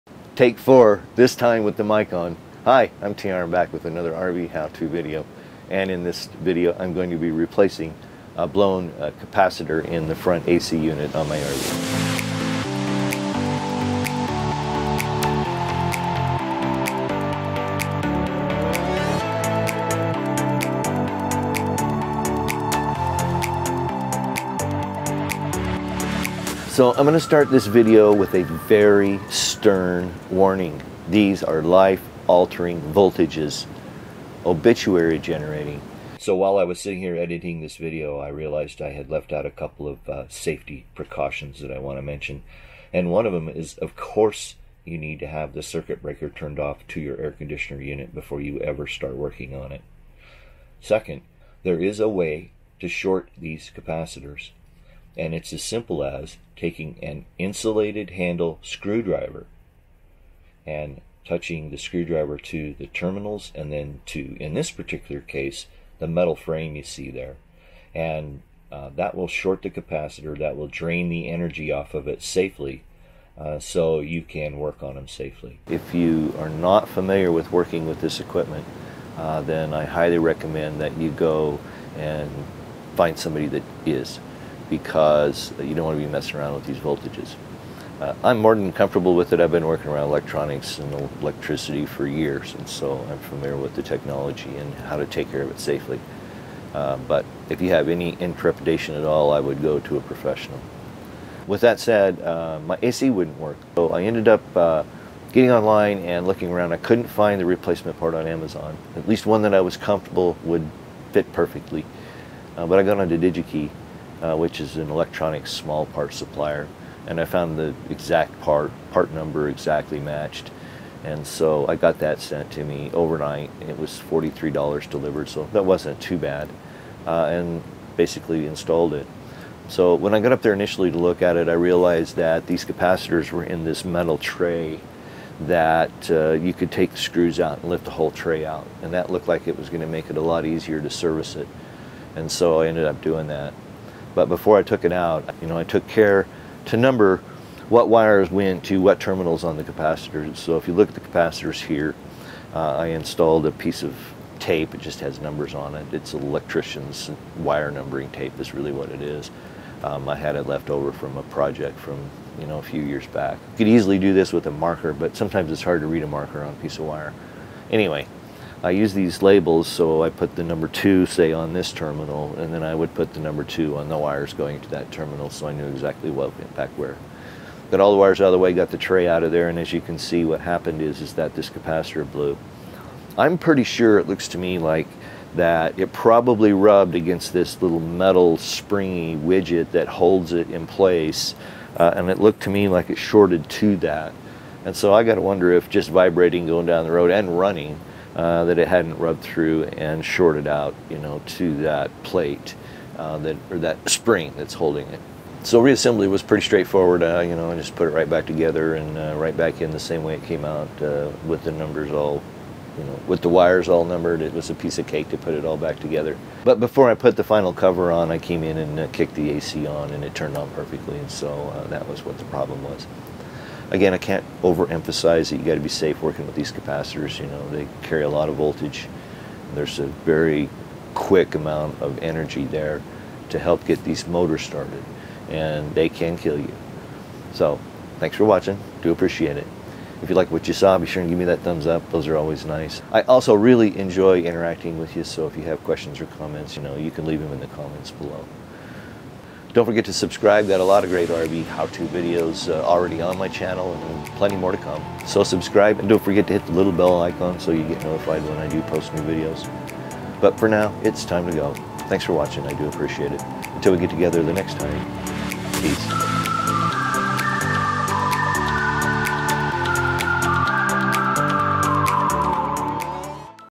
RV How To - Replacing a blown motor starter capacitor in my Air Conditioner
In this RV How To video I am replacing a blown motor starter capacitor in one of my Air Conditioning Units. It looks like the holding bracket must have worn through the outside wall and shorted the unit out.